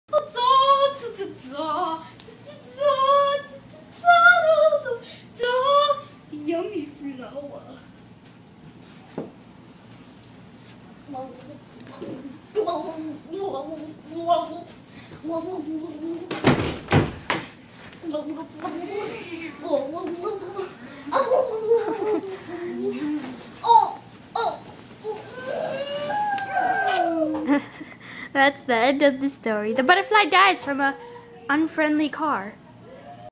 The Drunk Butterfly
A butterfly gets drunk by eating a strange looking fruit